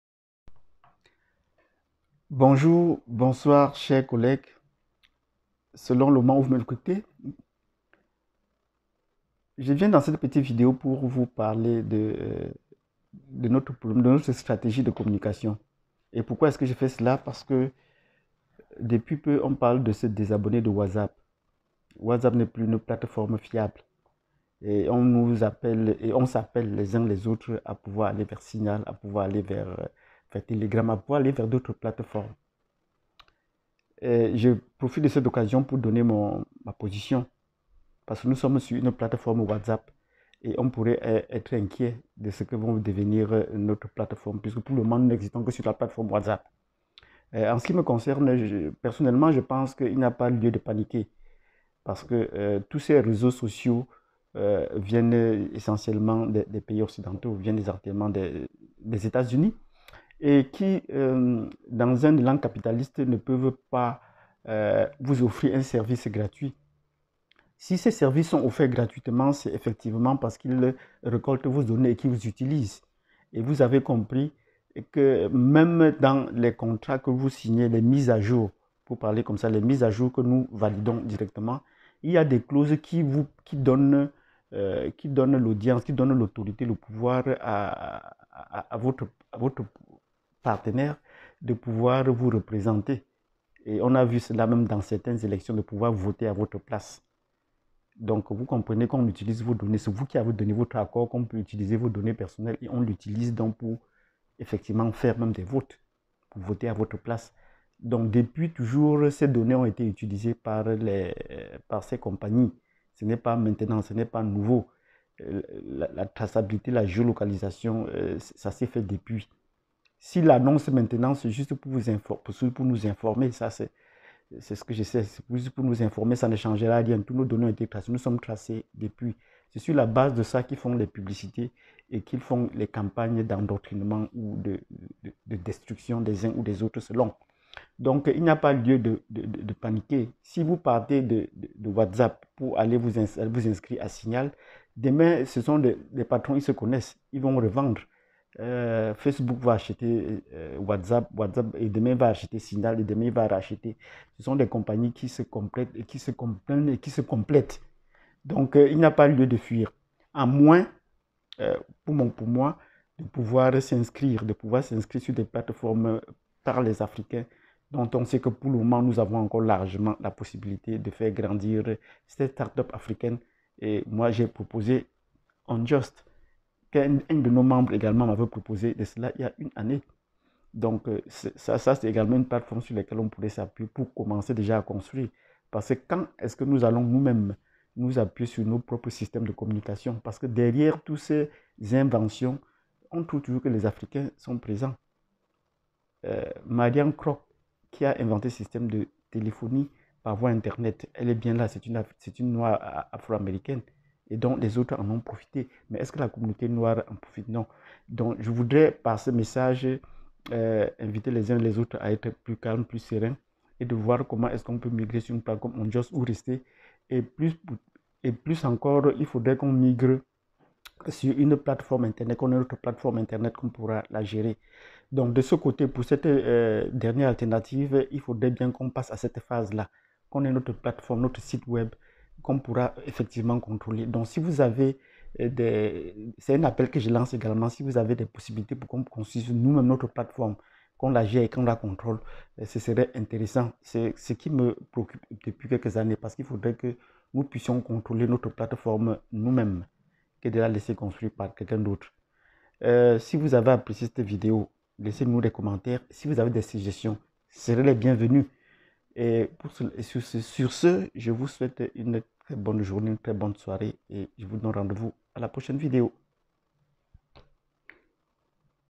Réseaux sociaux : Ne fuyez pas la pluie pour tomber dans l'eau
Changer de plateforme, par exemple de Whatsapp pour signal ou autres, n'est ce pas distribuer vous même vos coordonnées et données personnelles à ces capitalistes?
Quand vous migrer alors qu'on a toujours enregistrer sur vous .

Qu'en pensez-vous?